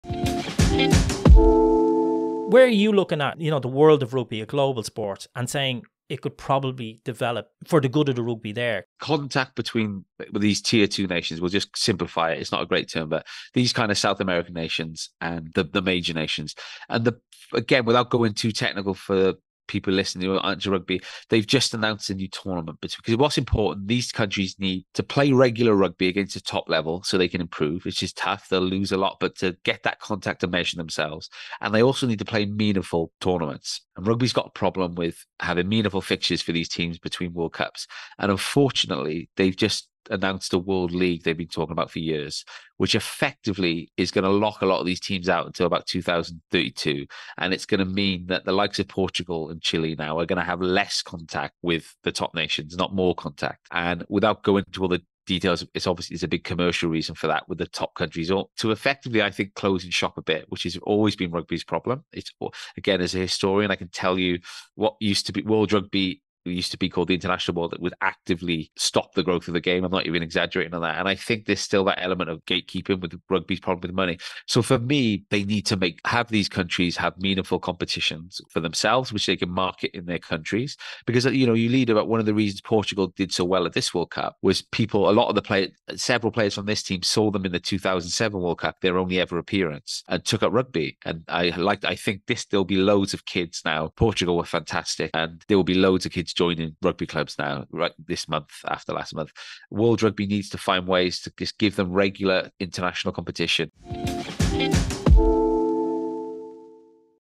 The Comfortable Spot with James Stafford
Today my guest is author and rugby historian James Stafford. A self confessed rugby fanatic, James is the founder of cult rugby website The East Terrace, and has written on rugby for publications such as The Daily Telegraph, Irish Independent, Metro, ESPN Scrum.com, and Rugby World. In 2017 his collection of short comics, The Sorrowful Putto of Prague, was published to critical acclaim in the Czech Republic and James now lives in Prague and coaches with Nyrsko RFC. I wanted to chat with James about his current opinion on the state of rugby worldwide, as well as his catalogue of books, so I hope you are sitting comfortably and happy to stay with us.